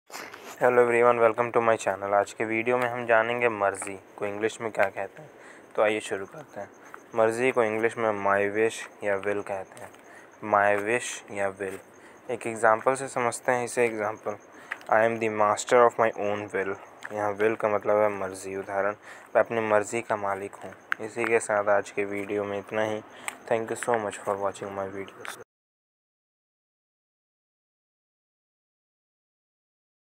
Marji ko english mein kya kahate hain | मर्ज़ी को इंग्लिश में क्या कहते है| My wish meaning in hindi
Marji ko English me kya kahate hain
Marji को इंग्लिश में क्या कहते है

मर्ज़ी को इंग्लिश में my wish कहते हैं
Marji ko English mein my wish kahate hain